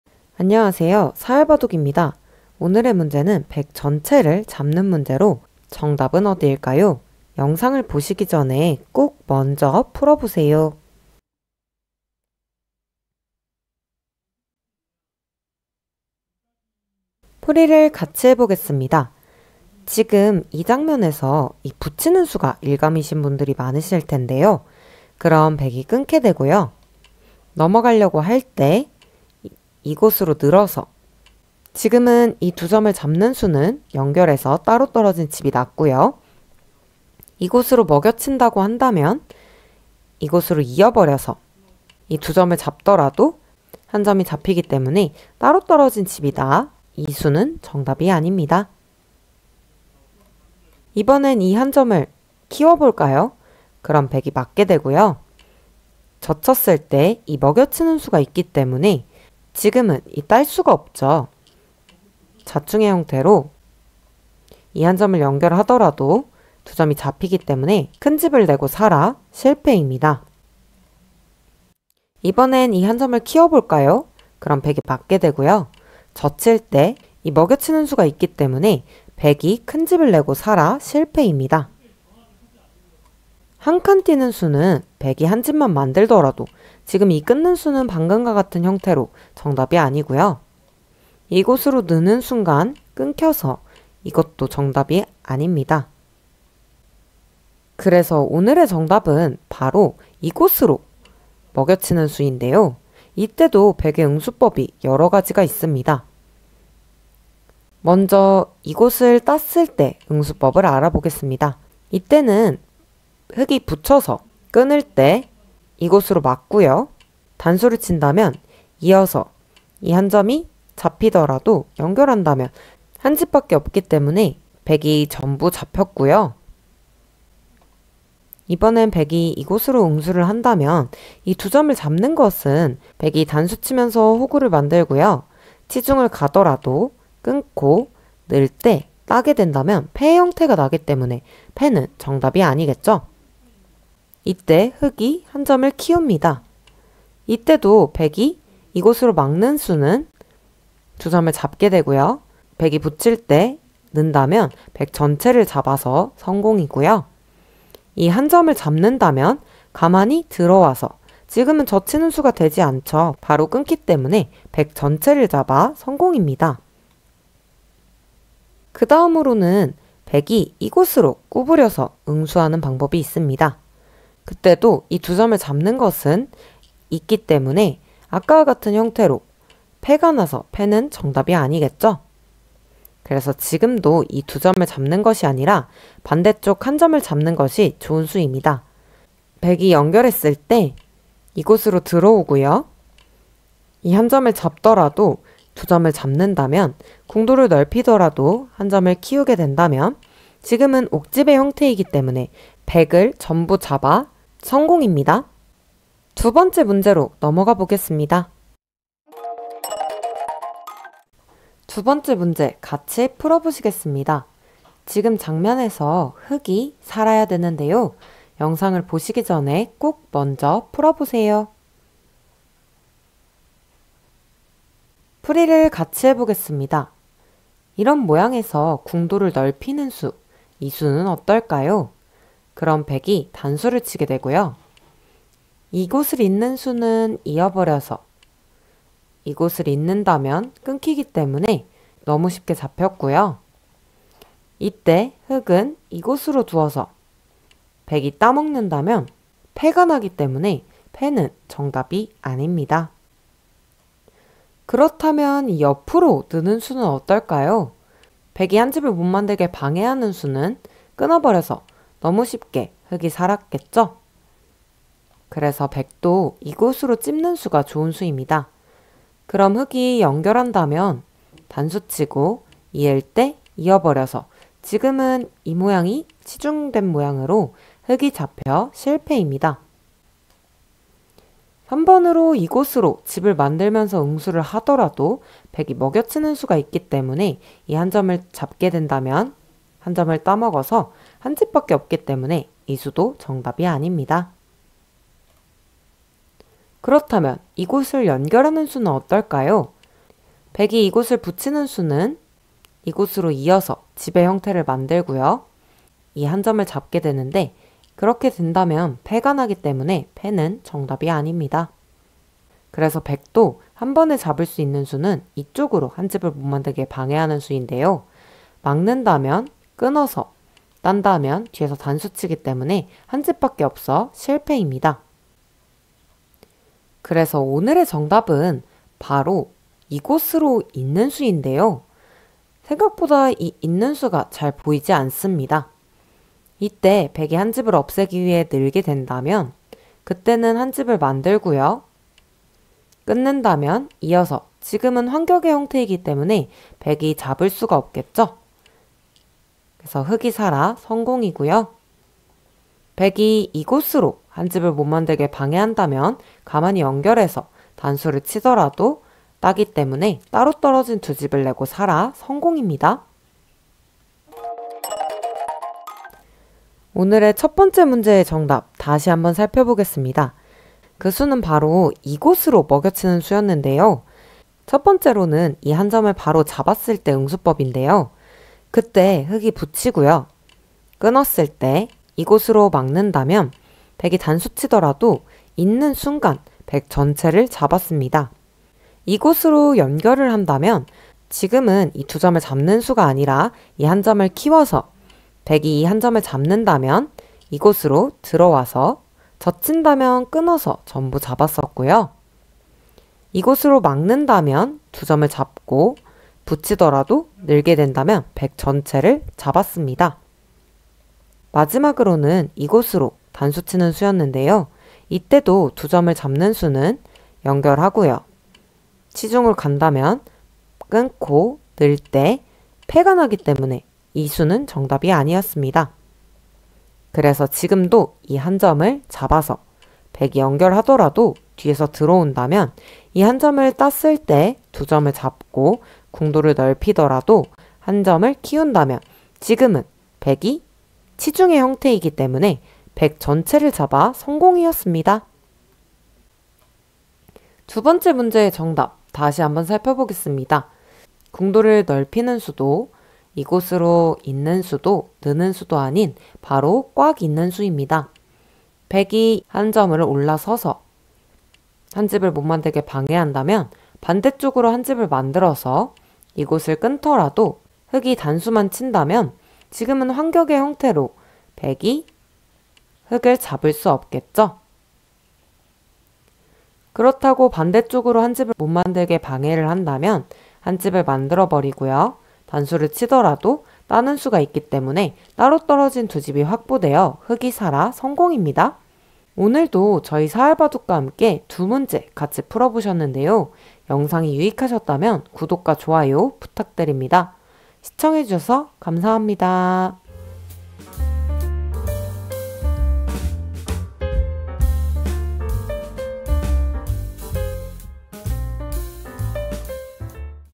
사활 바둑 묘수 - 이런 모양에 이런 수가? 백을 잡아보세요~
실전에서 많이 실수하는 문제입니다. 오늘도 저희 사활바둑과 함께 문제 풀어보시죠!